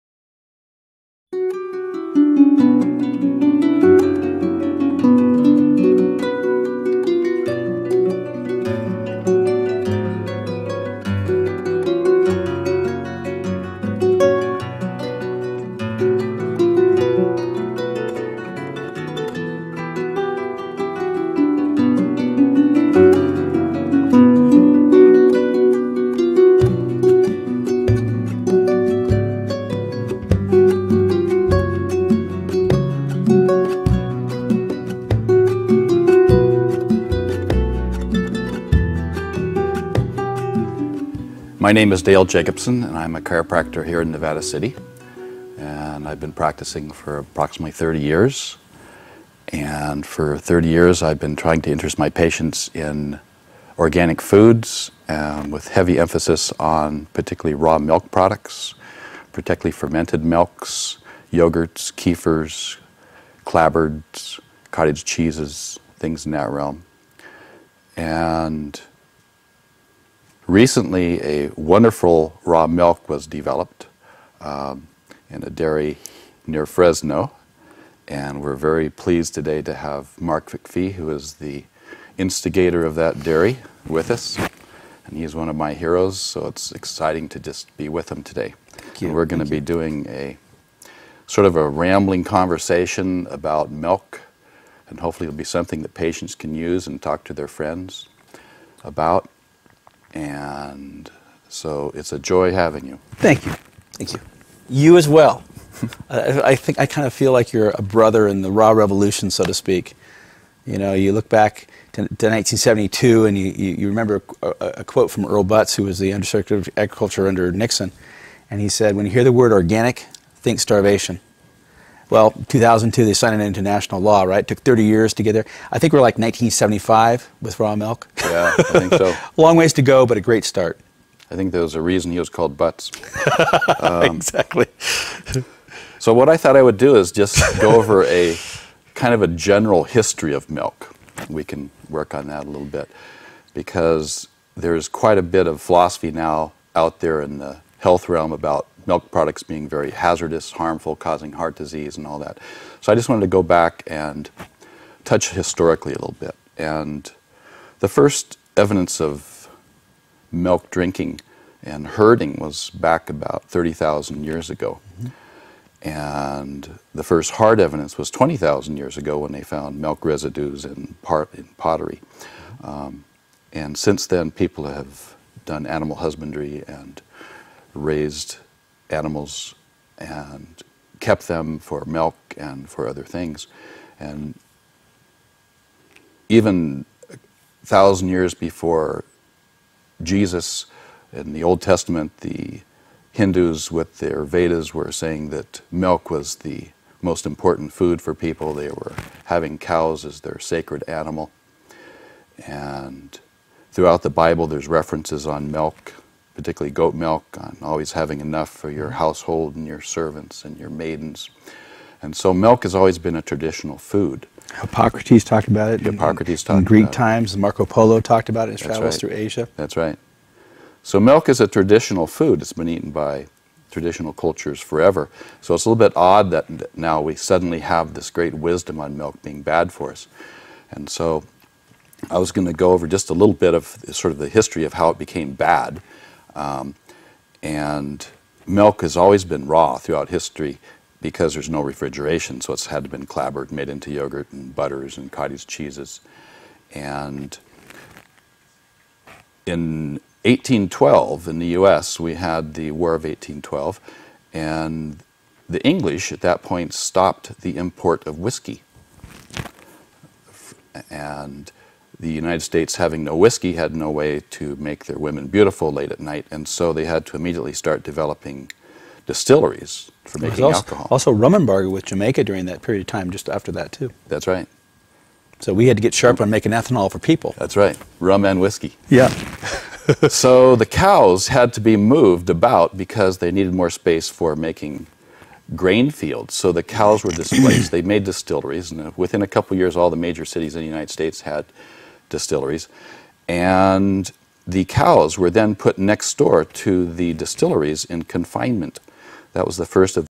The Raw Milk Controversy: Fact & Fiction Chapter 1, Prt. 1
An introduction to the conversation between Mark McAfee (founder & owner of Organic Pastures Dairy and Dr. Dale Jacobson, DC (Jacobson Chiropractic in Nevada City, California

For DVDs contact Chef Jem:
"chef at thesetruths dot com"